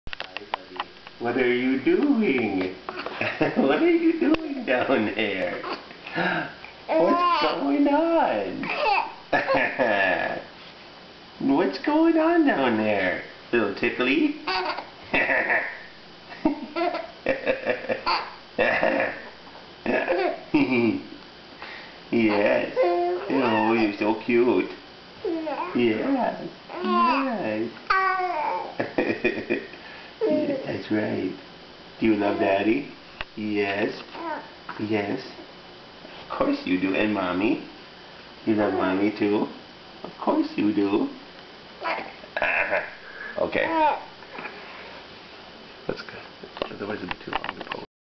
Giggling Isaac
Our first good video of Isaac laughing, smiling, and giggling! He usually will do this at the drop of a hat--UNTIL the camera comes out...I guess he's getting over being camera shy! He is 3 months old in this video.